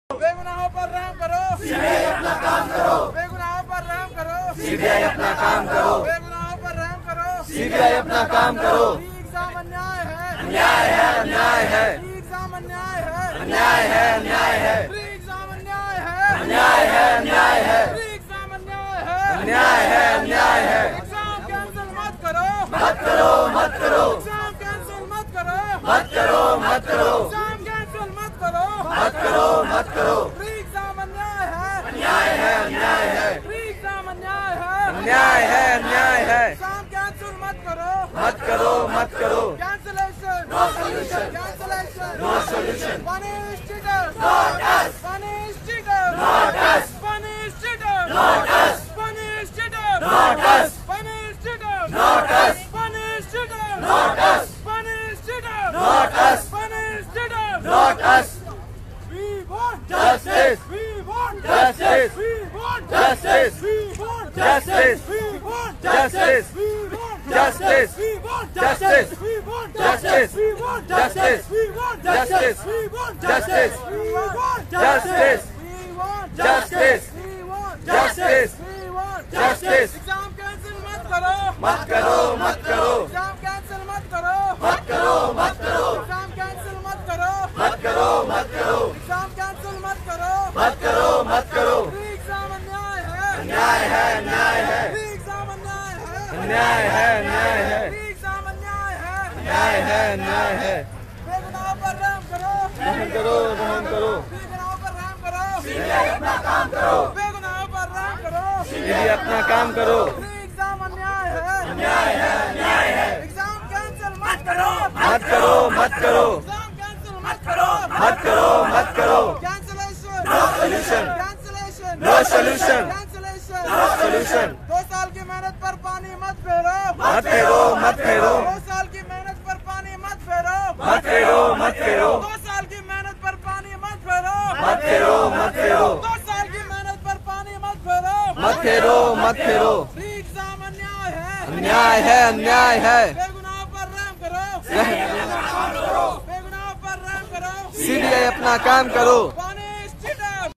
ssccgl Protest for NO Cancellation SSC CGL Exam (Recorded live) part-3 What U think?Y/N plz Comment
ssccgl Protest for NO Cancellation SSC CGL Exam (Recorded live) part-3 | candle March for SSC CGL NO Cancellation Exam
What You think ? Cancel exam hona chaye Ya Nhi
Plz Comment only Yes or NO